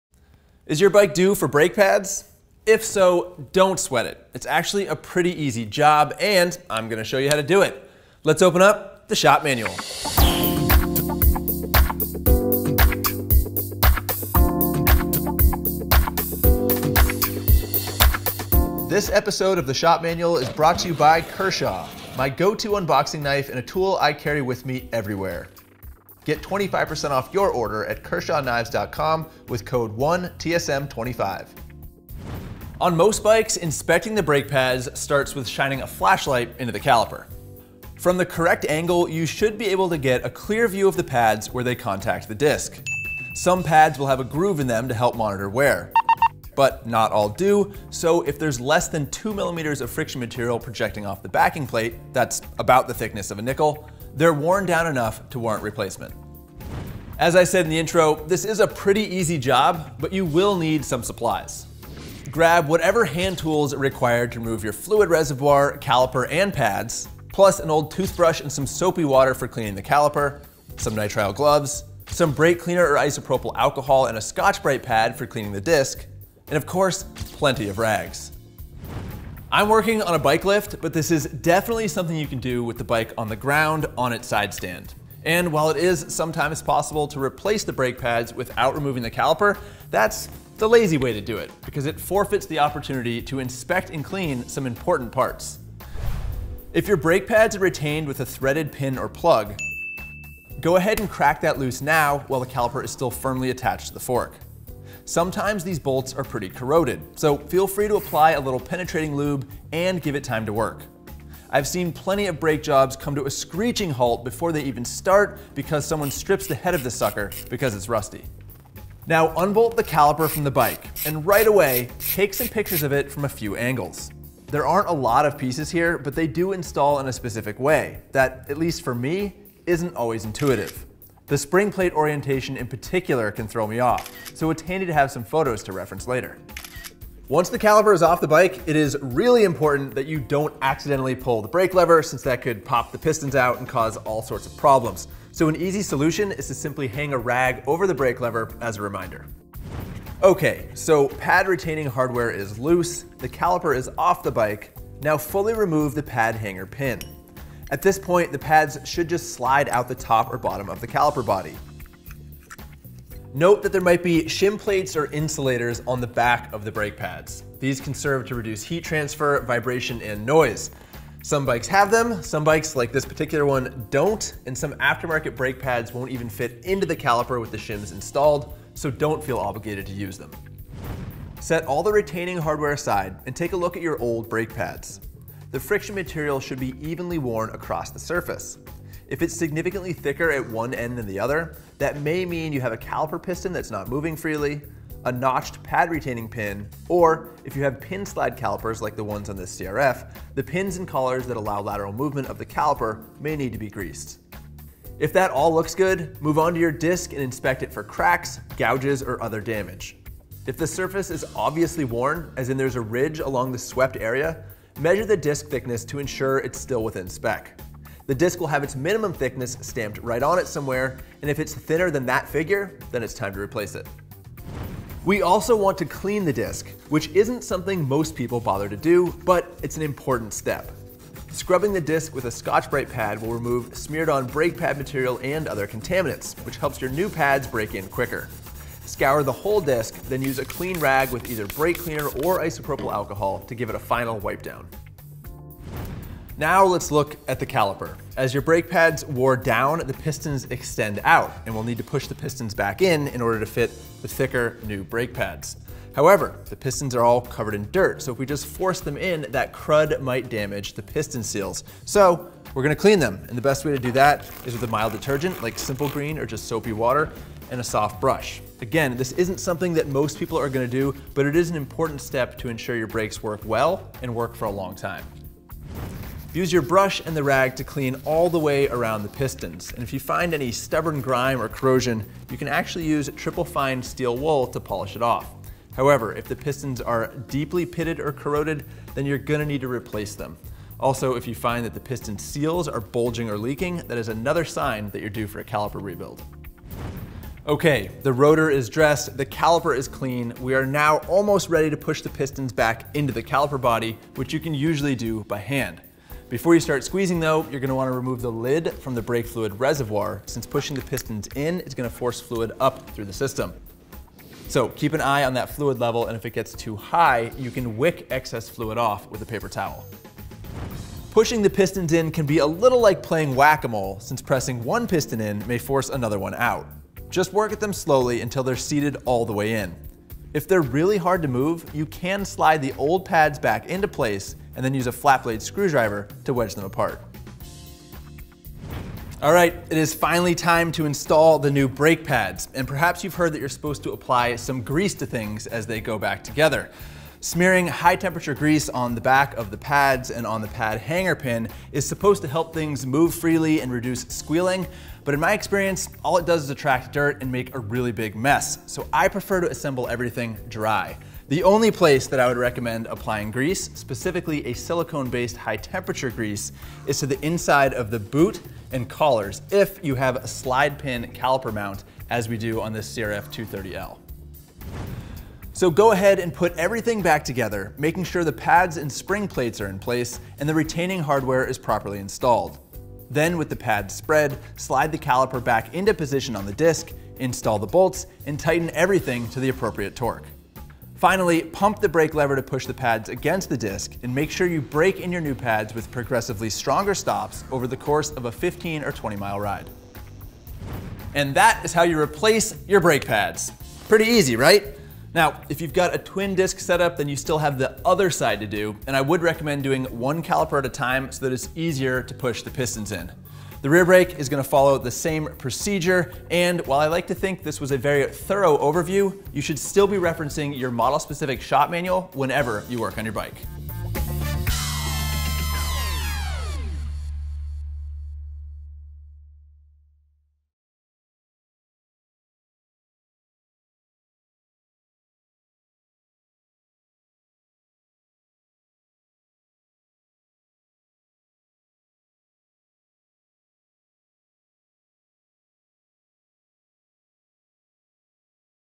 How To Replace Your Motorcycle Disc Brake Pads | The Shop Manual
Replacing your motorcycle’s brake pads is straightforward and fairly easy, especially if you’re just slapping in new pads. However, to keep your brakes working their best, there are a few extra steps you should take when servicing your brakes. In this episode of TSM, Ari shares tips and best practices for installing new brake pads and tuning up the entire brake system.